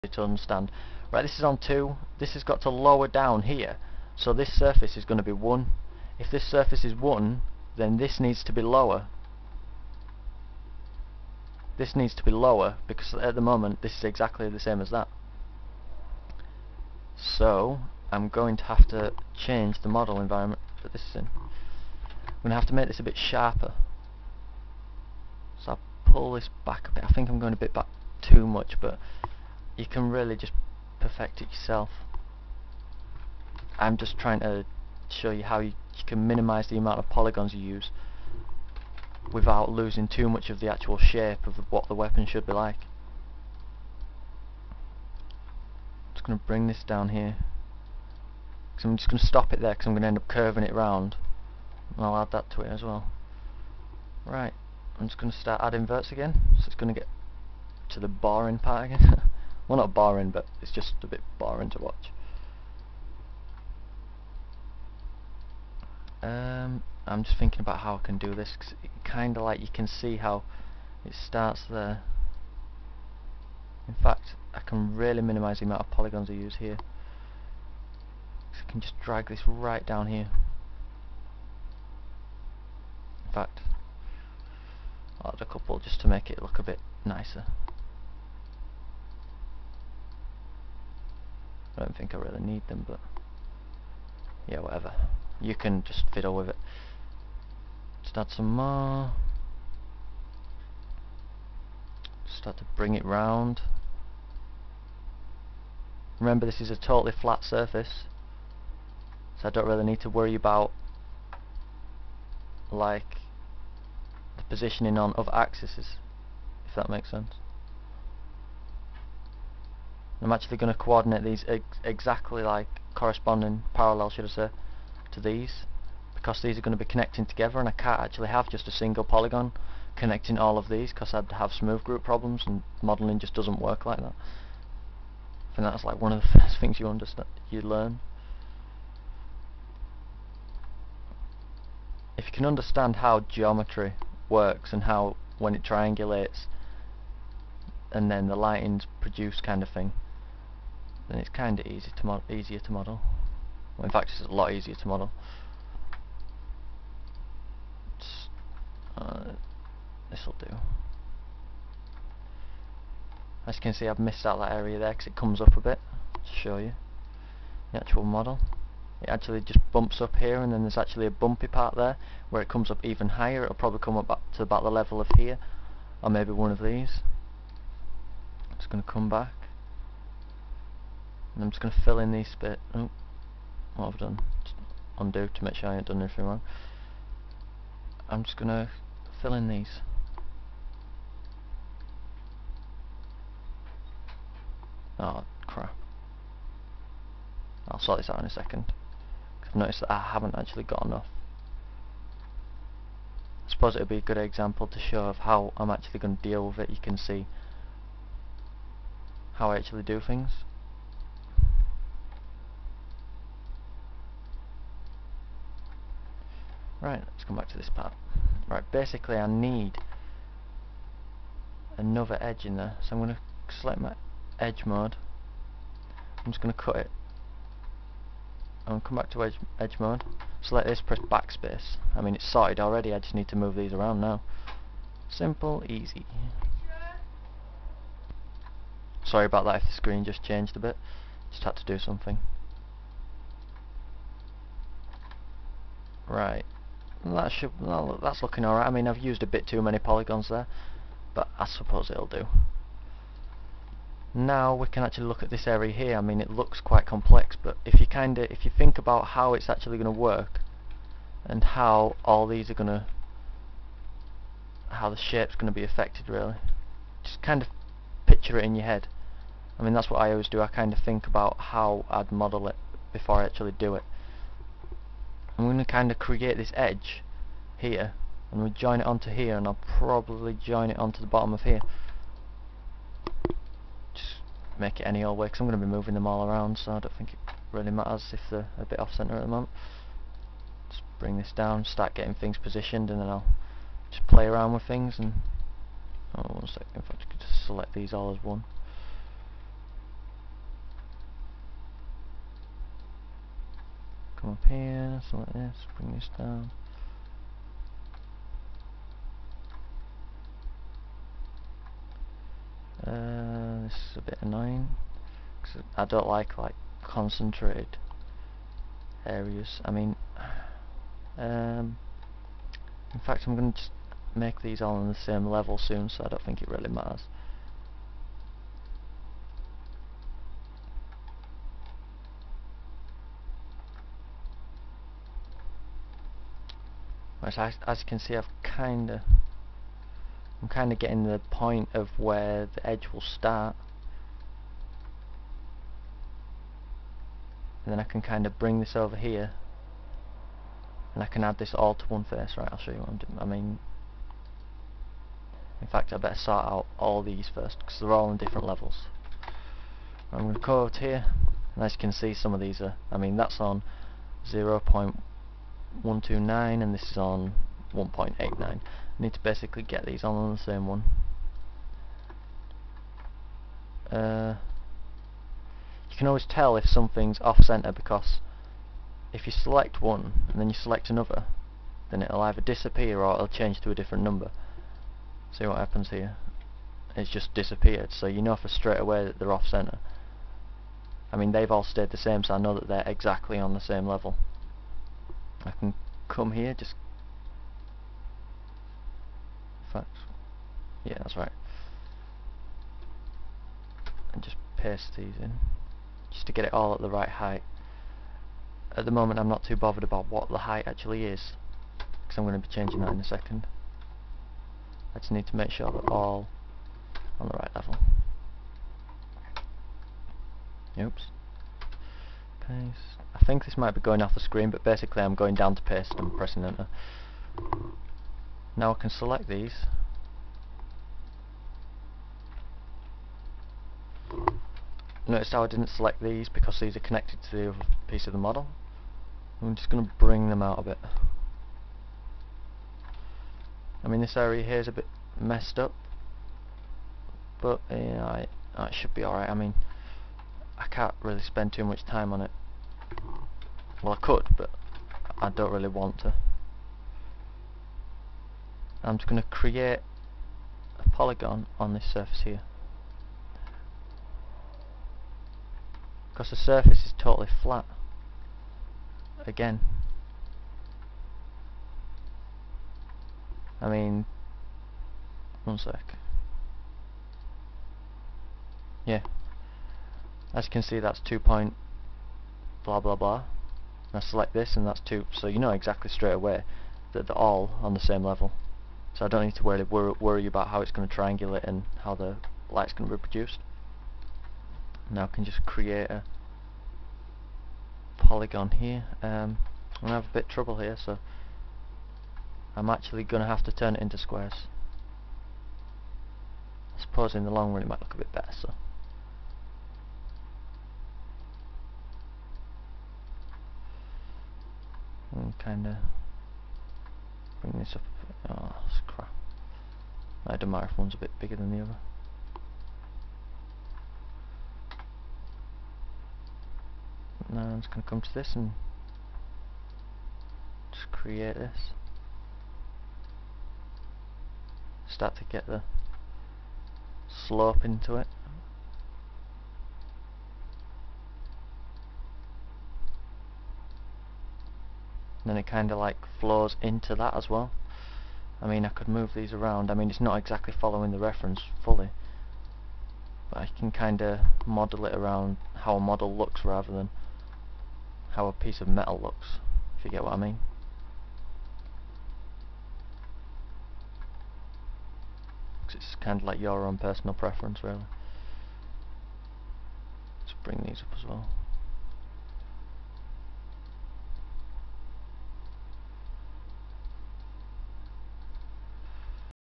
Beretta 9000 Modelling Tutorial - Part 2 (3 of 5) - Modelling Base
This is the second part of the Beretta 9000 modelling tutorial made by Ichi. In the video, he is using the program 3d Studio Max.

The second part is actually 46 minutes (approximately), so I split it into 5 parts. This is part 3 of 5.